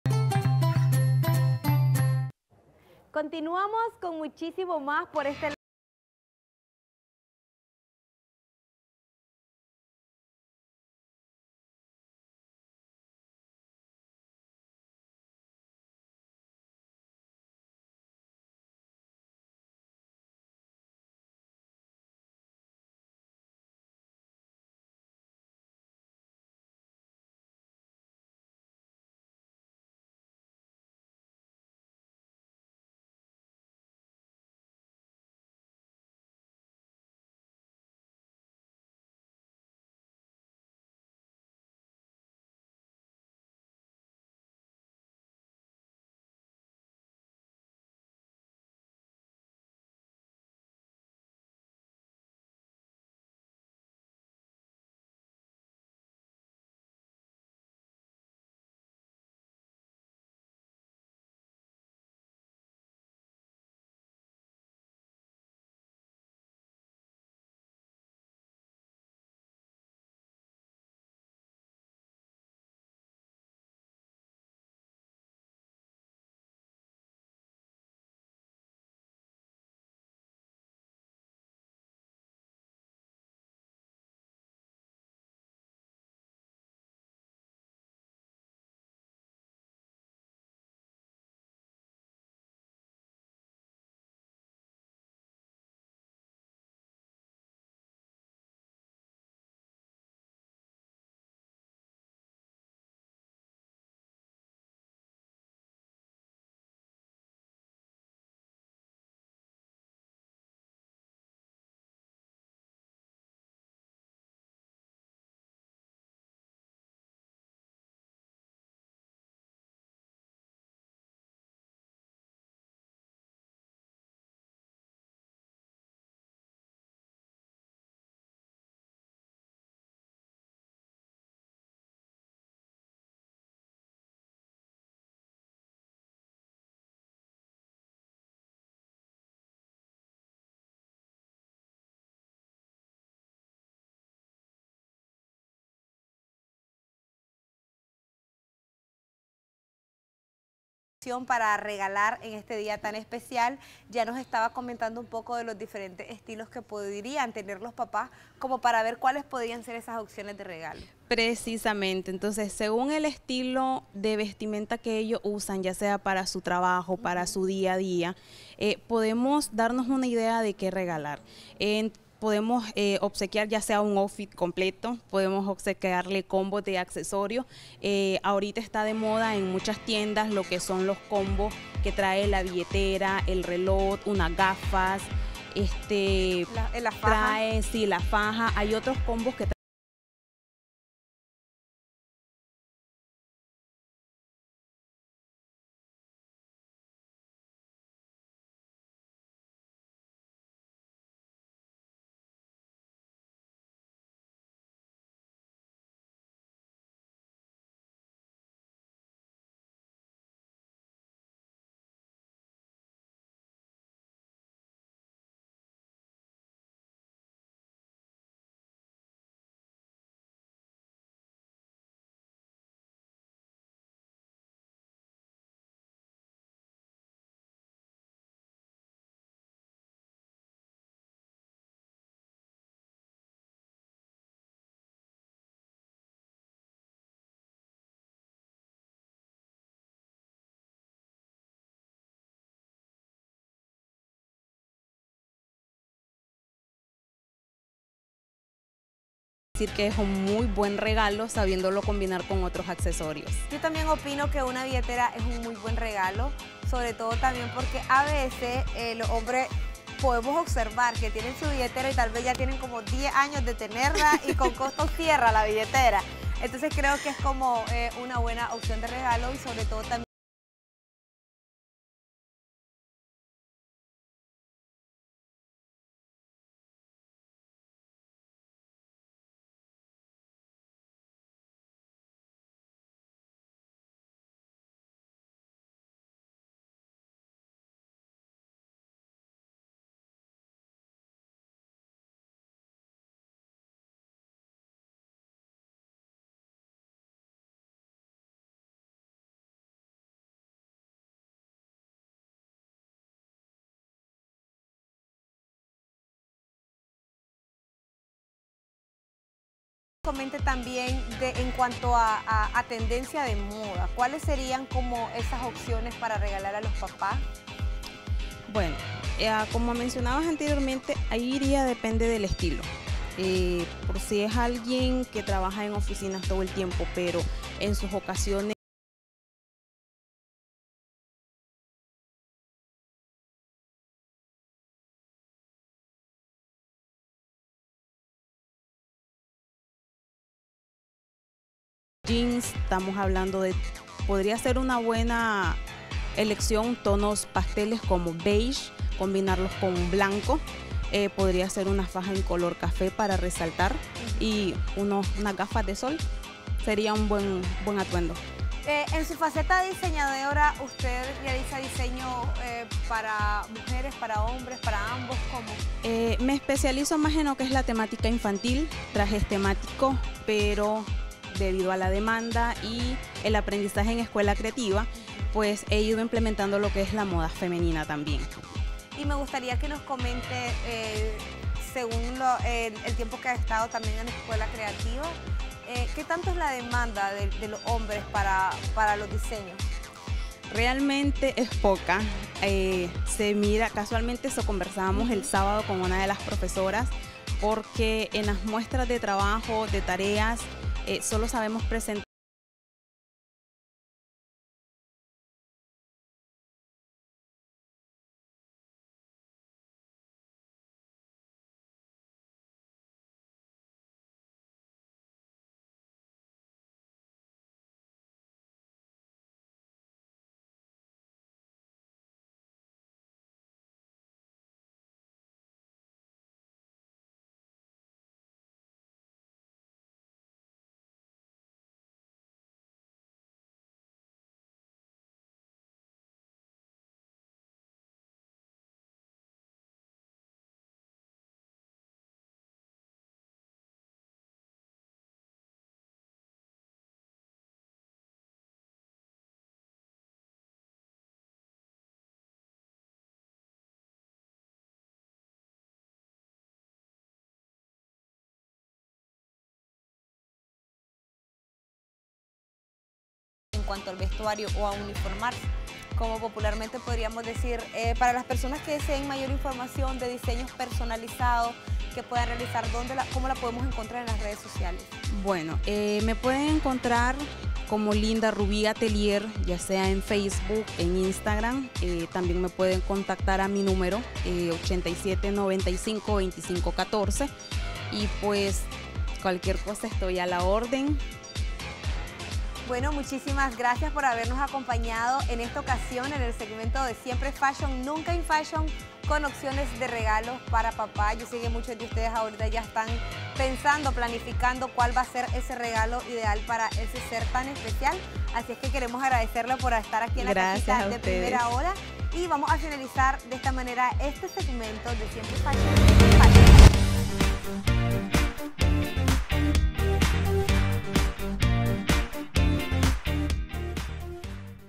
Opciones de regalos para papá en su día especial
En el segmento siempre Fashion nunca Infashion nos acompañó la diseñadora Linda Rubí, de la Escuela Creativa, la cual nos comparte las mejores opciones de regalos para los hijos que deseen dar un detalle bonito a sus padres, de cara a la celebración del padre nicaragüense.